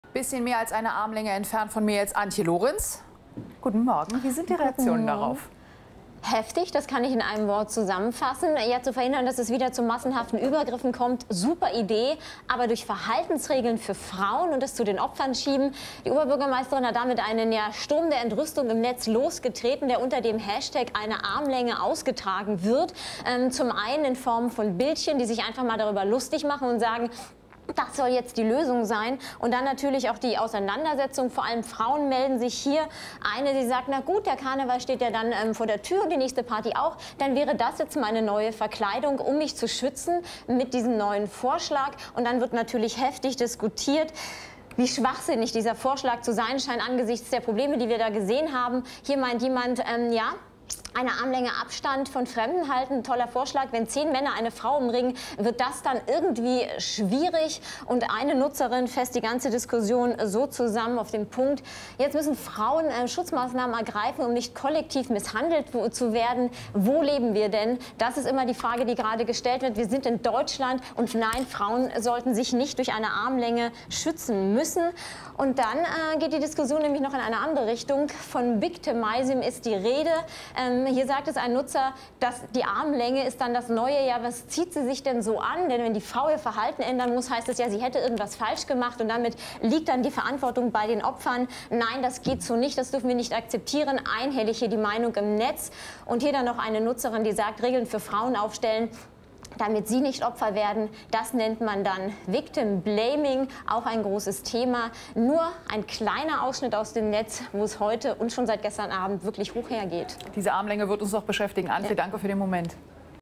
EineArmlaenge Abstand: Oberbürgermeisterin von Köln entrüstet mit Verhaltensregeln für Frauen
Nach den massenhaften Übergriffen auf Frauen in Köln in der Silvesternacht schlägt Köln Oberbürgermeisterin Henriette Reker Verhaltensregeln für Frauen vor - zu deren eigenem Schutz. Zum Beispiel sollten sie eine Armlänge Abstand von Fremden halten. Im Netz ist ein Sturm der Entrüstung losgebrochen. Damit schiebe man die Verantwortung zu den Opfern, die Meinungen sind einhellig.